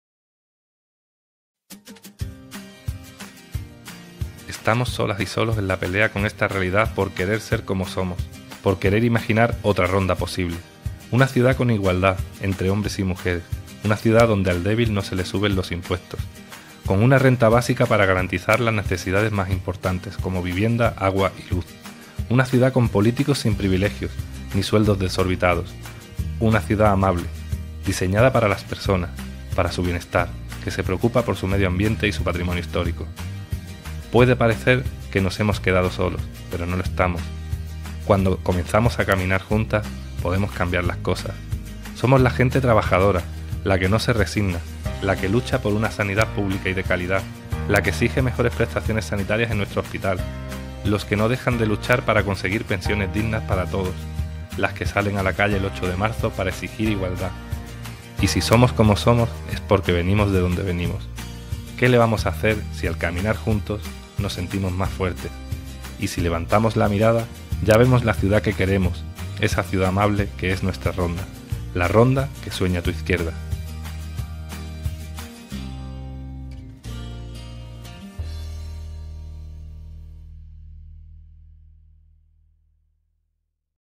Spot Electoral IU Ronda. Álvaro Carreño. Ciudad Amable.
Spot Electoral de IU Ronda para las elecciones municipales de 2019.